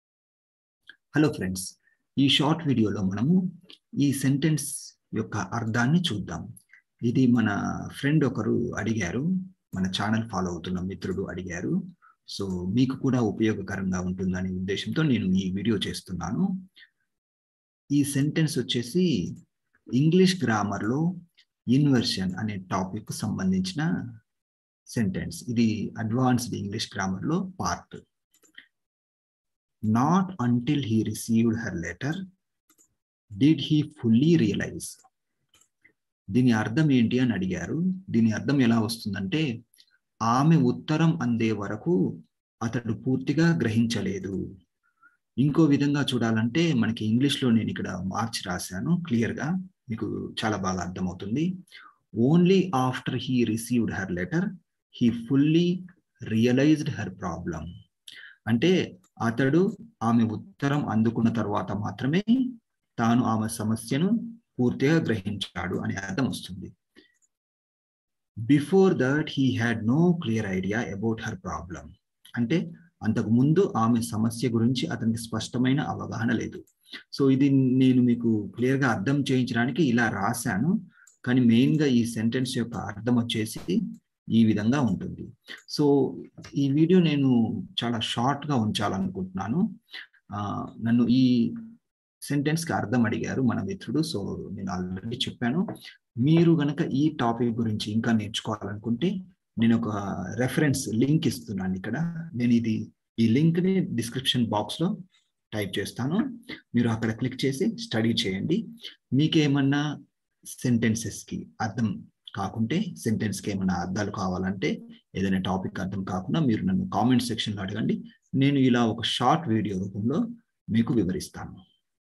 Learn English through Telugu | Inversion in English Grammar - Example Sentences
Not until he received her letter did he fully realize. = ఆమె ఉత్తరం అందే వరకు అతడు పూర్తిగా గ్రహించలేదు. | Explanation: Only after he received her letter, he fully realized her problem. Before that he had no clear idea about her problem. = అతడు ఆమె ఉత్తరం అందుకున్న తర్వాత మాత్రమే, తాను ఆమె సమస్యను పూర్తిగా గ్రహించాడు. అంతకు ముందు ఆమె సమస్య గురించి అతనికి స్పష్టమైన అవగాహన లేదు.

Presented by Praveen Ragi, PR Learning Lab